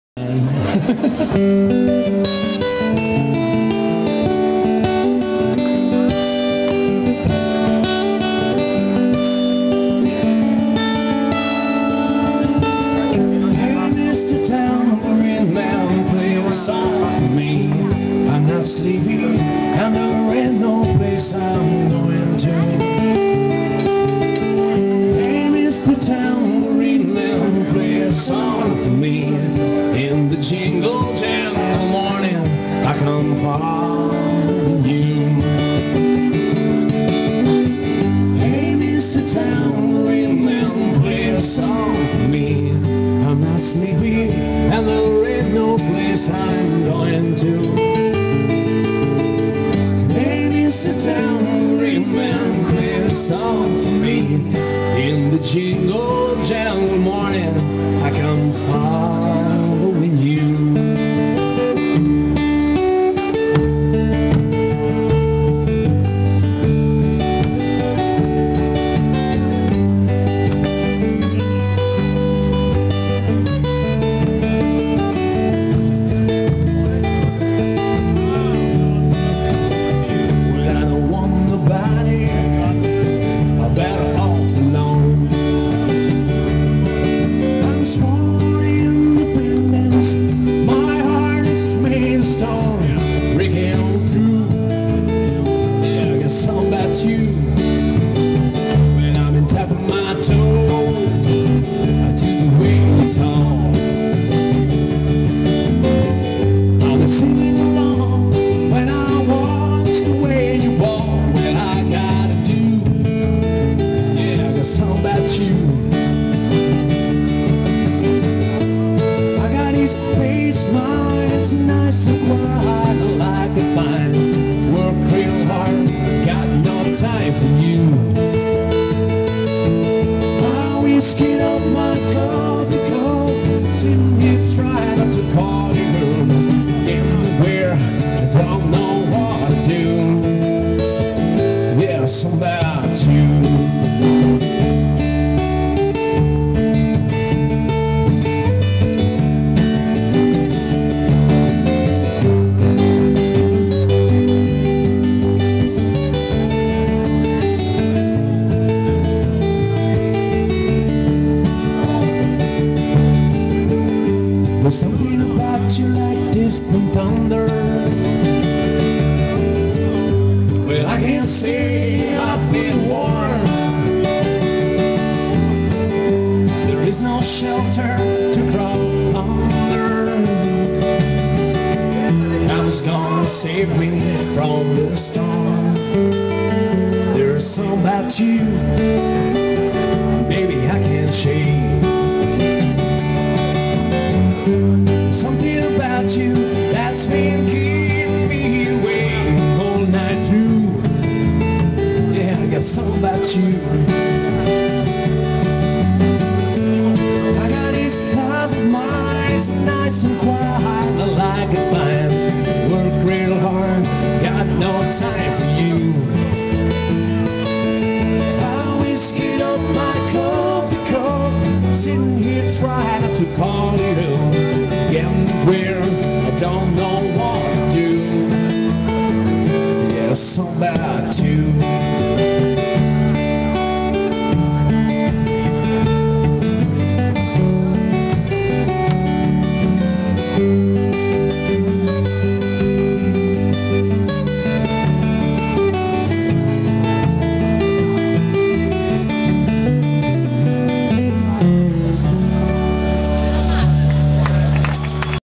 Beach Garden Music Concert
QuattroMenumQuarto are playing music for people in Beach Garden - Marina di Campo - Elba Island (Italy) 31 July 2008